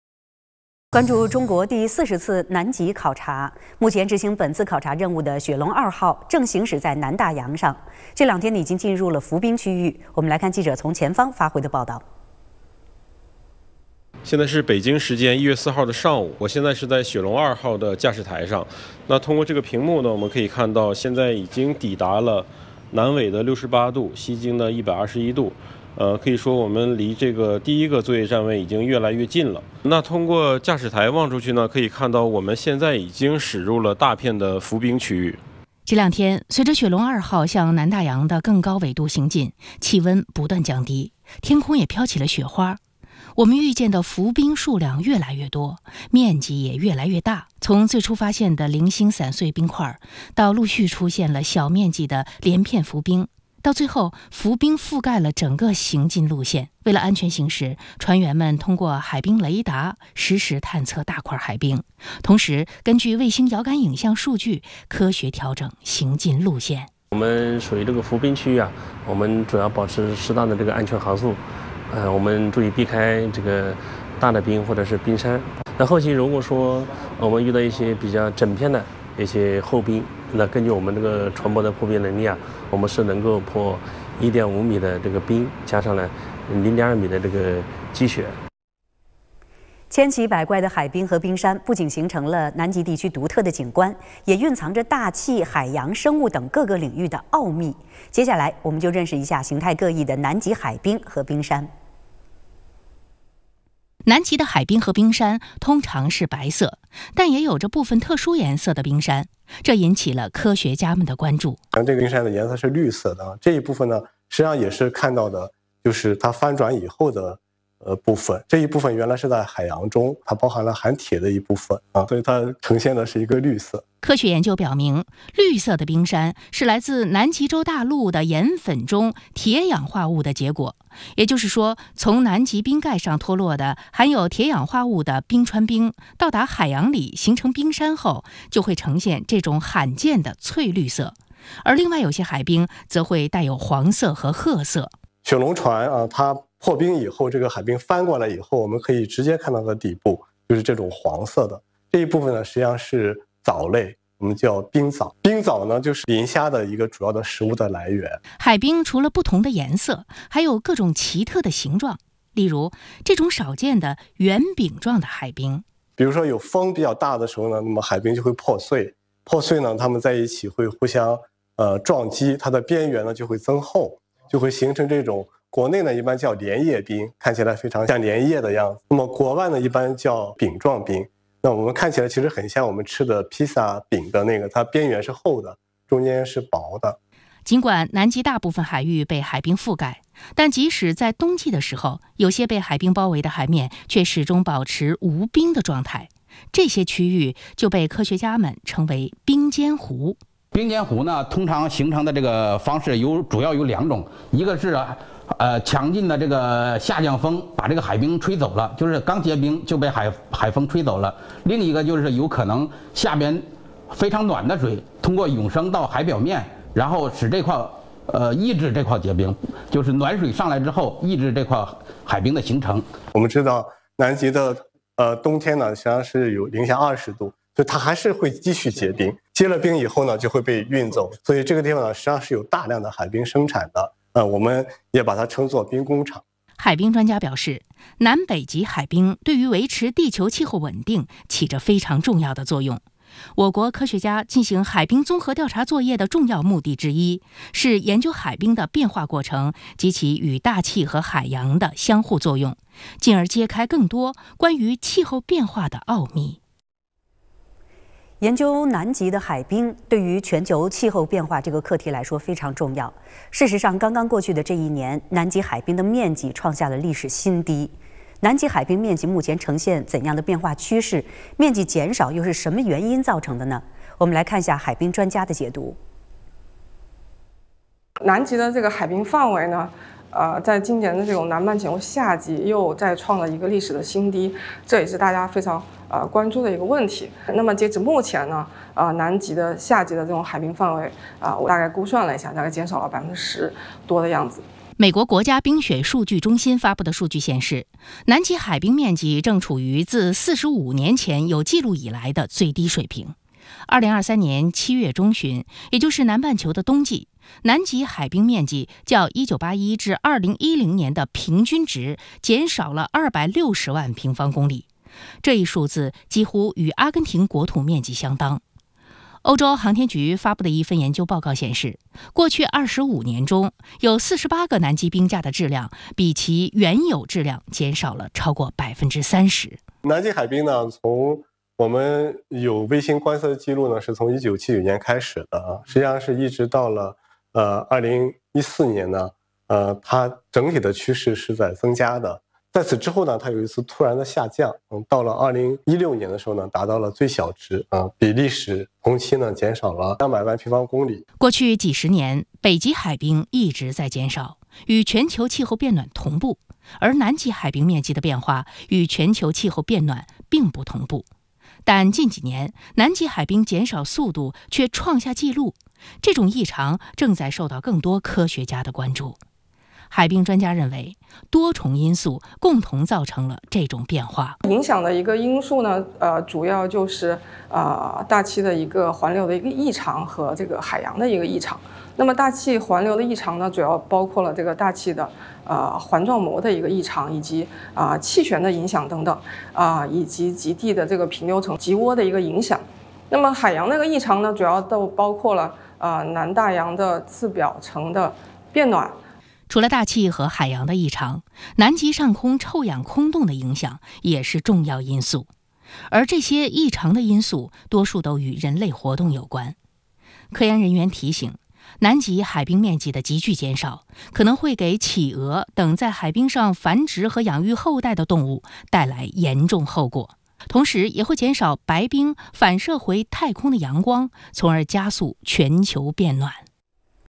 中国第40次南极考察：“雪龙2”号驶入浮冰区
关注中国第40次南极考察。目前执行本次考察任务的“雪龙2”号，正行驶在南大洋上，这两天已经进入了浮冰区域。

总台央视记者 陈博：现在是北京时间1月4日的上午，我现在是在“雪龙2”号的驾驶台上，通过这个屏幕可以看到，现在已经抵达了南纬68度，西经121度，可以说我们离第一个作业站位已经越来越近了。通过驾驶台望出去可以看到，我们现在已经驶入了大片的浮冰区域。

这两天，随着向南大洋的更高纬度行进，气温不断降低，天空也飘起了雪花。“雪龙2”号遇见的浮冰数量越来越多、面积越来越大。从最初发现的零星散碎冰块，到陆续出现了小面积的连片浮冰，到最后浮冰覆盖了整个行进路线。为了安全行驶，船员们通过海冰雷达实时探测大块海冰，同时根据卫星遥感影像数据科学调整行进路线。

“雪龙2”号驾驶员 肖金峰：现在属于浮冰区，我们主要保持适当的安全航速，注意避开大的冰或者冰山。后期如果说我们遇到整片的厚冰，根据我们的破冰能力，是能够破1.5米厚度的冰，加0.2米厚度的积雪。

南极的海冰和冰山通常是白色，但也有着部分特殊颜色的冰山，引起科学家们的更大关注。

中国海洋大学教授 史久新：像这个冰山的颜色是绿色的，这一部分实际上看到的就是它翻转以后的部分，这一部分原来是在海洋中，因为它包含了含铁的一部分，所以它呈现的是绿色。

科学研究表明，绿色的冰山是来自南极洲大陆的岩粉中“铁氧化物”的结果，也就是说，从南极冰盖上脱落的含有“铁氧化物”的冰川冰，到达海洋里形成冰山后，就会呈现这种罕见的翠绿色。而另外有些海冰则会带有黄色和褐色。比如雪龙船破冰以后，海冰翻过来可以看到它的底部是黄色的，这一部分实际上是冰藻，冰藻是磷虾的一个主要的食物来源。

海冰除了不同的颜色，还有各种奇特的形状，也值得科学家们进行研究，例如这种少见的圆饼状的海冰。

中国海洋大学教授 史久新：风比较大的时候，海冰就会破碎，它们在一块会互相撞击，边缘就会增厚，形成这种冰。国内一般叫莲叶冰，看起来非常像莲叶的样子。国外一般叫饼状冰，看起来很像比萨饼。它的边缘是厚的，中间是薄的。

研究南极的海冰，对于全球气候变化这个课题来说非常重要。事实上，刚刚过去的这一年南极海冰面积创下了历史新低。南极海冰面积目前呈现怎样的变化趋势？面积减少又是什么原因造成的？

武汉大学中国南极测绘研究中心教授 刘婷婷：截至目前，南极夏季的海冰范围，大致估算，与2015—2016年比大概减少了10%以上。

美国国家冰雪数据中心发布的数据显示，南极海冰面积正处于自45年前有记录以来的最低水平。2023年7月中旬也就是南半球的冬季，南极海冰面积较1981—2010年的平均值减少了260万平方公里，这一数字几乎与阿根廷国土面积相当。欧洲航天局发布的一份研究报告显示，过去25年中，有48个南极冰架的质量比其原有质量减少了超过30%。

过去几十年，北极海冰一直在减少，与全球气候变暖同步，而南极海冰面积的变化，与全球气候变暖并不同步。但近几年南极海冰减少速度却创下纪录，这种异常正在受到更多科学家的关注。海冰专家认为，多重因素共同造成这种变化。

武汉大学中国南极测绘研究中心教授 刘婷婷：影响的一个因素，主要就是大气环流的异常和海洋的异常。大气环流的异常，主要包括了大气的环状模的异常以及气旋的影响等，以及极地平流层的极涡的一个影响，海洋异常主要包括了南大洋的次表层的变暖。

除了大气和海洋的异常，南极上空臭氧空洞的影响也是重要因素。而这些异常的因素，多数都与人类活动有关。科研人员提醒，南极海冰面积的急剧减少，可能会给企鹅等在海冰上繁殖和养育后代的动物带来严重后果，同时也会减少白冰反射回太空的阳光，从而加速全球变暖。